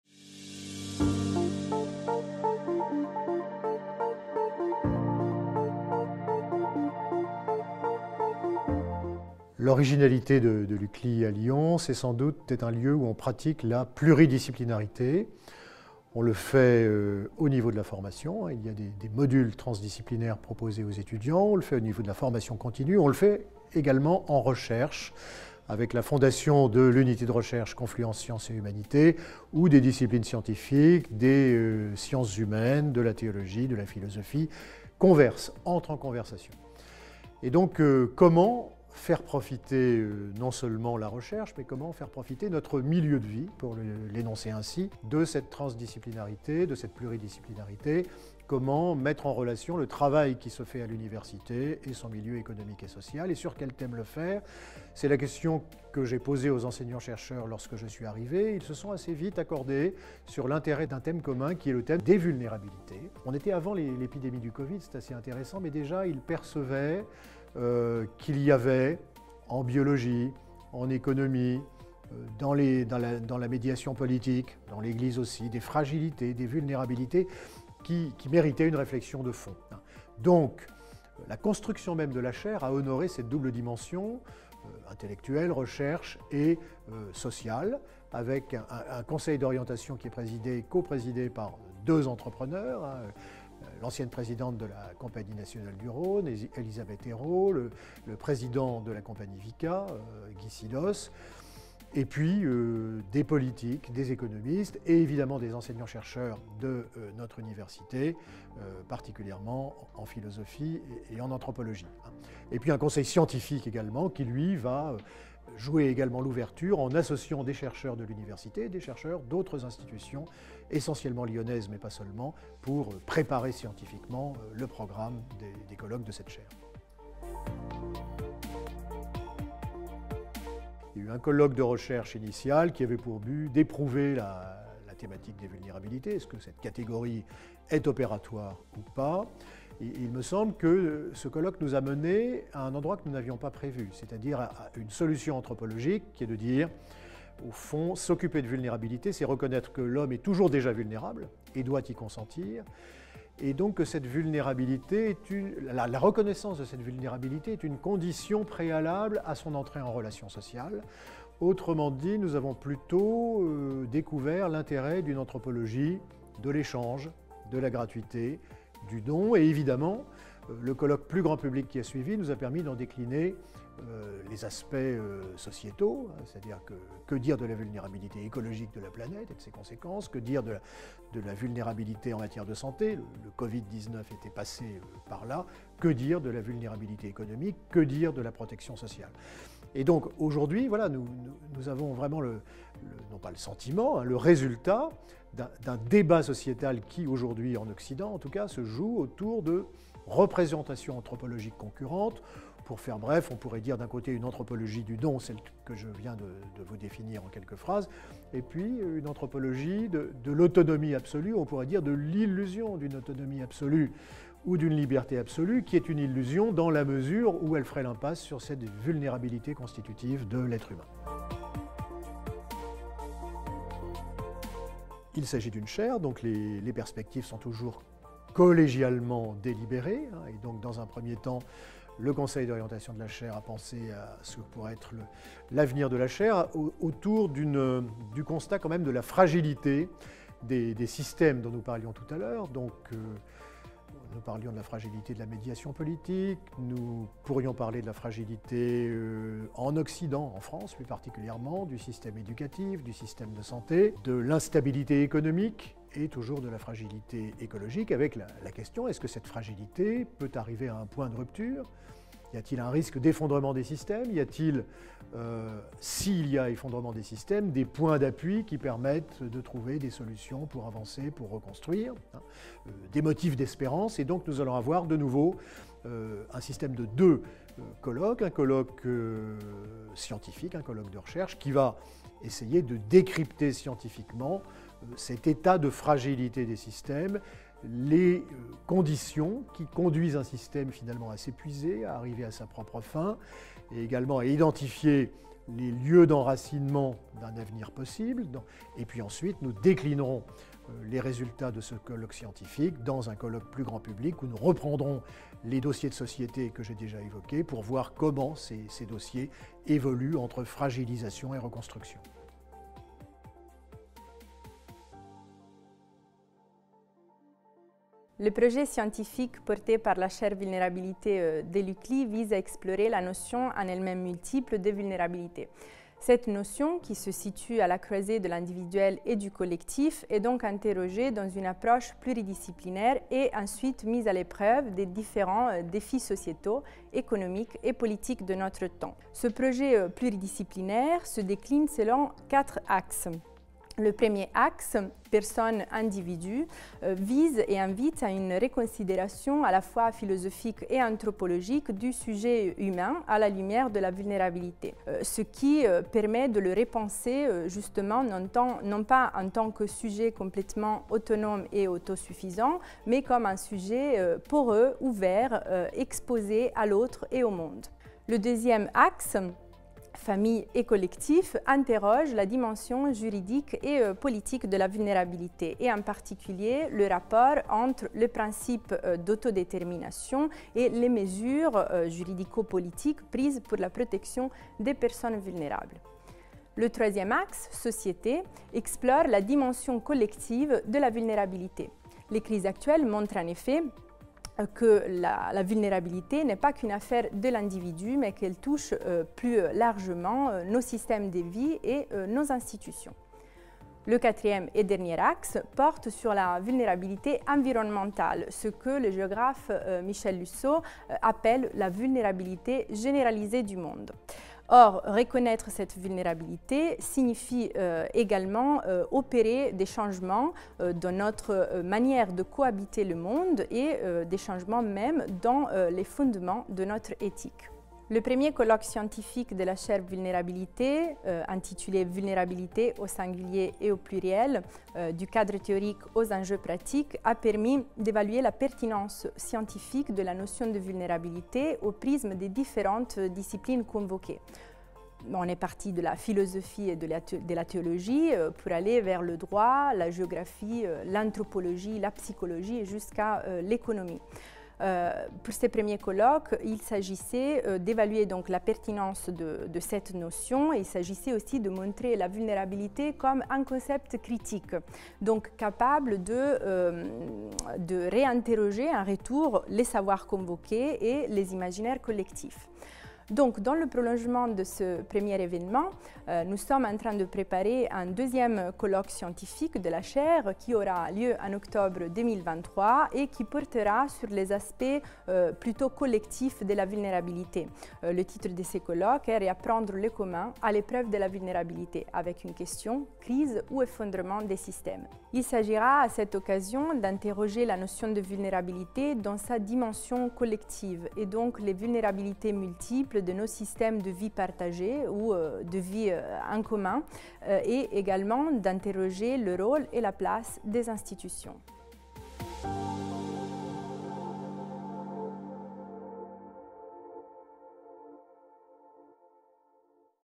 Olivier Artus et Chiara Pesaresi présentent la Chaire d’Université Vulnérabilités de l'UCLy
Au cours de ces entretiens, le Professeur Olivier Artus, Recteur de l’UCLy, expose la raison d’être de la Chaire d’Université Vulnérabilités, de sa vision intellectuelle à son apport sociétal. La parole est ensuite donnée à Chiara Pesaresi, philosophe et directrice scientifique de la Chaire, qui s’exprime sur les grands axes de réflexion, cœur des activités et des travaux de ce «laboratoire d’idées ». En savoir plus sur la Chaire Vulnérabilités